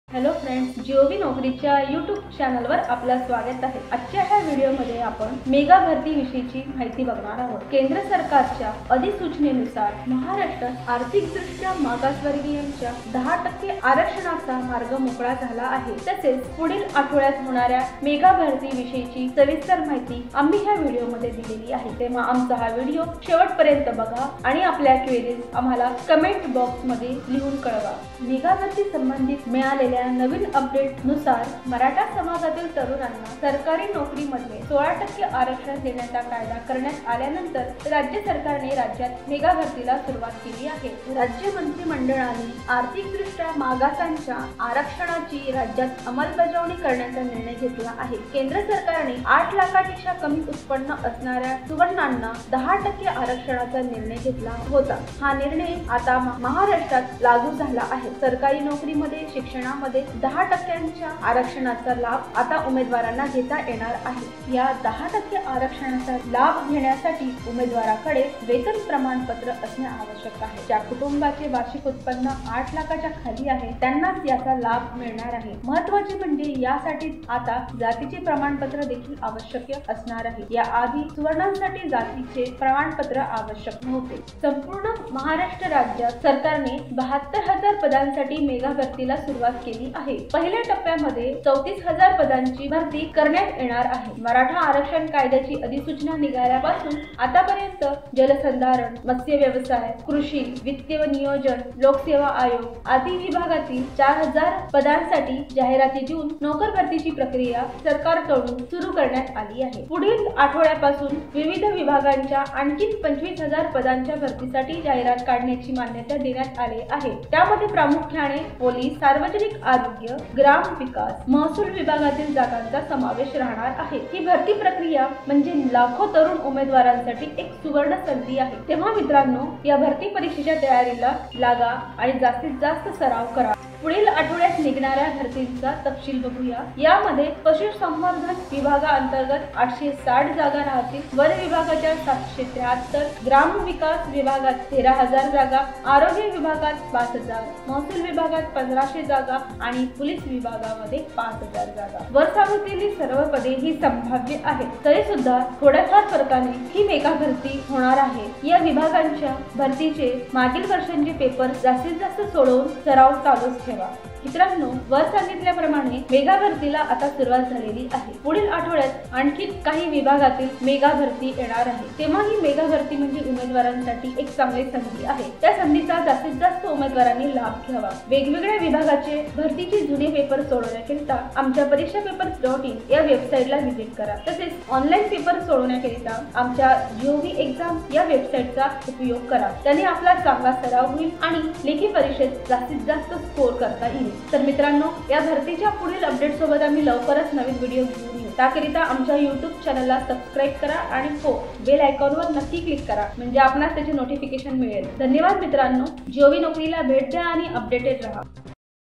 72000 Mega Bharti 2019 Latest update
Mega Bharti 2019 in Maharashtra has starting now. Latest updates regarding the reservation and mega bharti is available here.